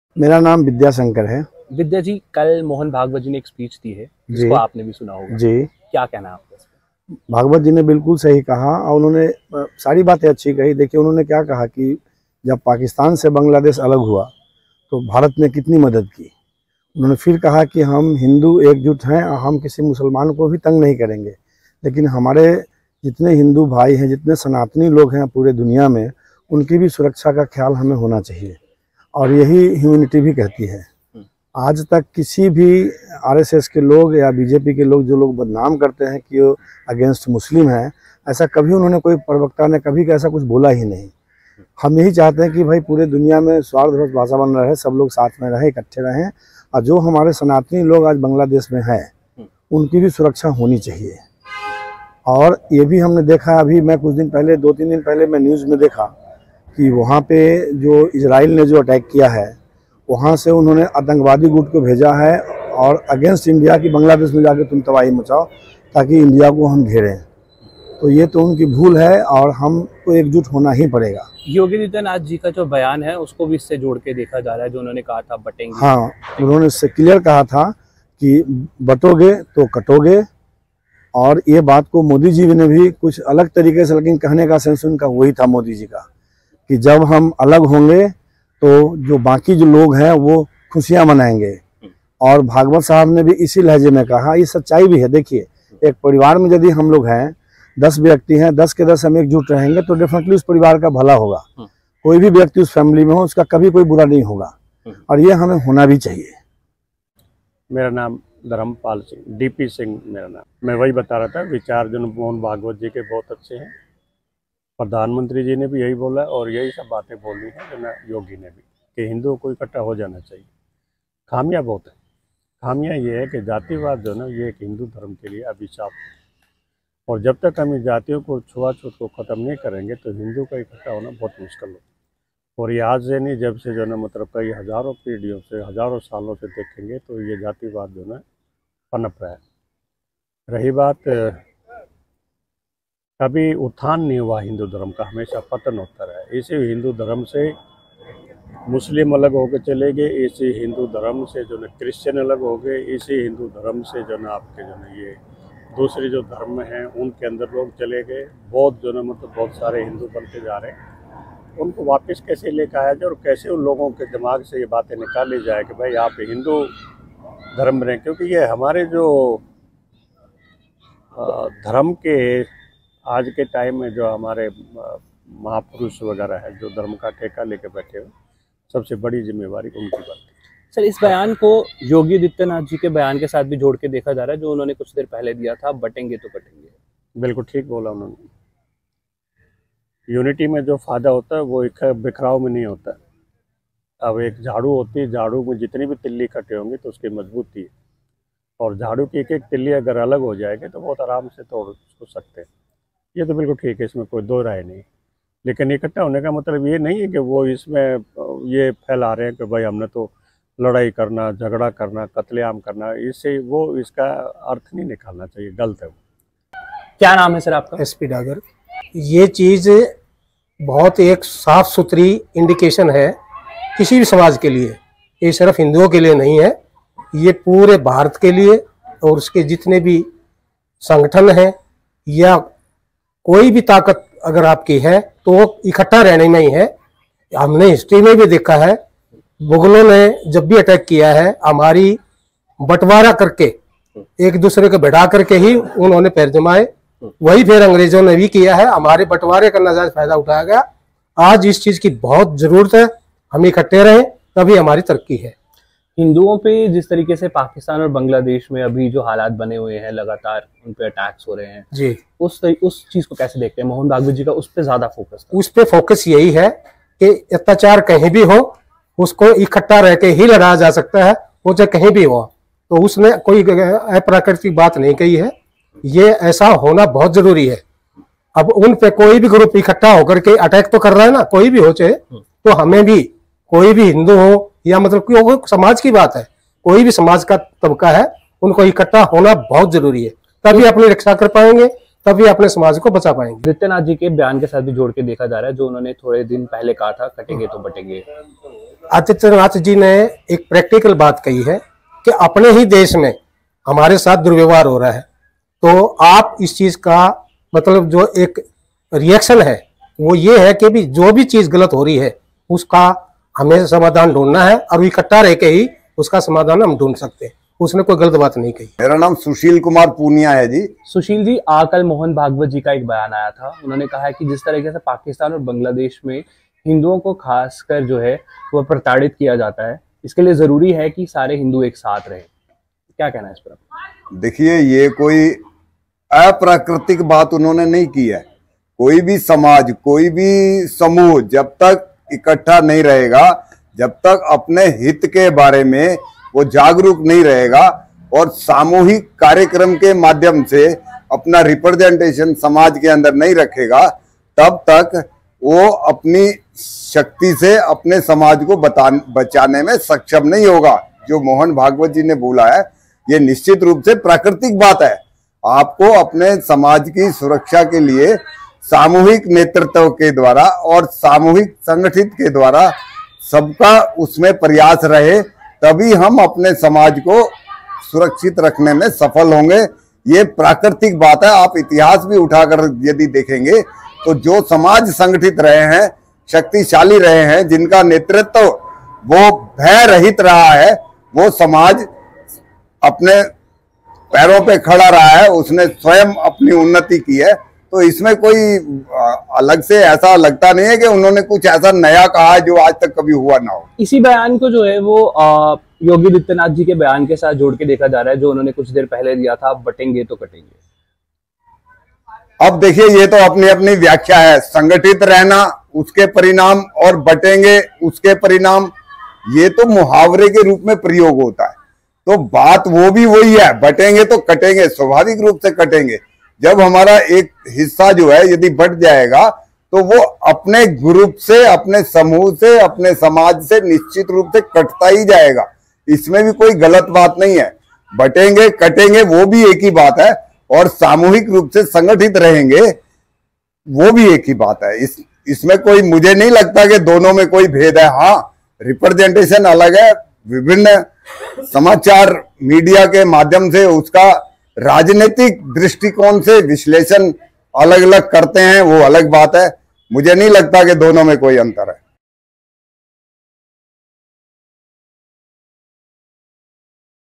CM Yogi और Bhagwat के बयान पर जनता की राय, समर्थन या विरोध?
On the occasion of Dussehra, Rashtriya Swayamsevak Sangh (RSS) chief Mohan Bhagwat spoke about the atrocities being committed against Hindus worldwide. He specifically expressed concern over the continuous persecution of minority Hindus in Bangladesh and Pakistan. Recently, Uttar Pradesh Chief Minister Yogi Adityanath also addressed the issue of atrocities against Hindus in Bangladesh, during which he used the phrase "batenge to katenge" (if divided, there will be bloodshed), which is now being linked to his earlier statement.
कल दशहरे के मौके पर राष्ट्रीय स्वयंसेवक संघ के प्रमुख मोहन भागवत ने दुनिया में हिंदुओं के खिलाफ हो रहे अत्याचार का जिक्र किया. खासकर उन्होंने बांग्लादेश में और पाकिस्तान में हो रहे अल्पसंख्यक हिंदुओं पर लगातार अत्याचार पर भी अपनी चिंता जताई. वहीं हाल ही में उत्तर प्रदेश के मुख्यमंत्री योगी आदित्यनाथ ने भी जब बांग्लादेश के हिंदुओं पर हो रहे अत्याचार पर अपना भाषण दिया था तो तब उन्होंने 'बटेंगे तो कटेंगे' जैसे जुमले का भी इस्तेमाल किया था जिसे अब इस बयान से भी जोड़कर देखा जा रहा है.